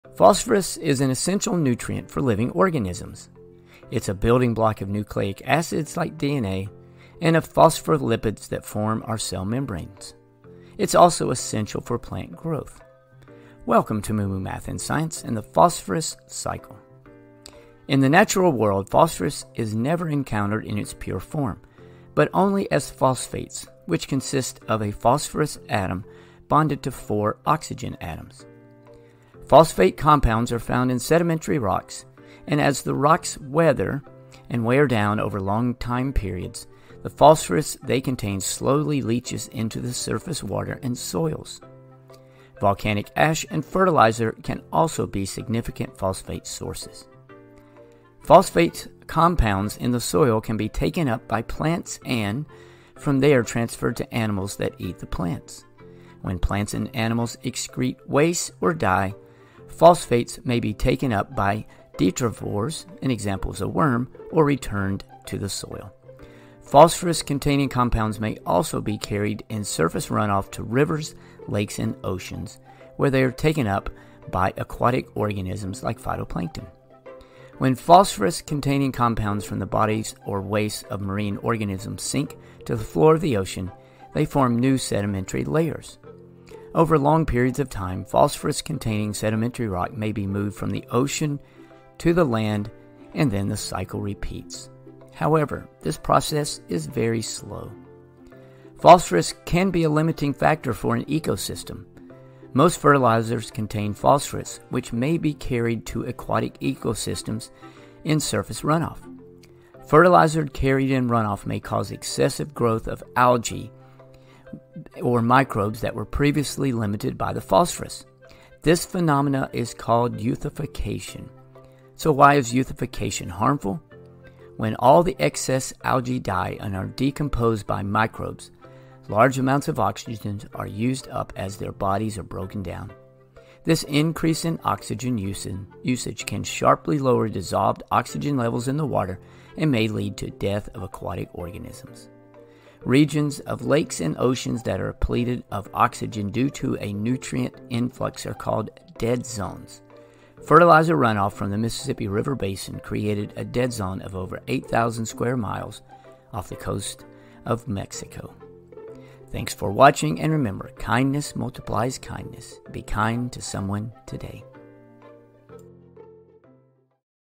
Phosphorus Cycle
Learn about the Phosphorus cycle. Phosphorus is an essential nutrient for living organisms. It’s a building block of nucleic acids, like  DNA, and phospholipids that form our cell membranes. It is also essential for plant growth.
In the natural world phosphorous is never encountered in its pure form, but only as phosphates, which consists of a phosphorus atom bonded to four oxygen atoms
Phosphate compounds are found in sedimentary rocks, and as the rocks weather—wear down over long time periods—the phosphorus they contain slowly leaches into surface water and soils. Volcanic ash and fertilizer can also be significant phosphate sources.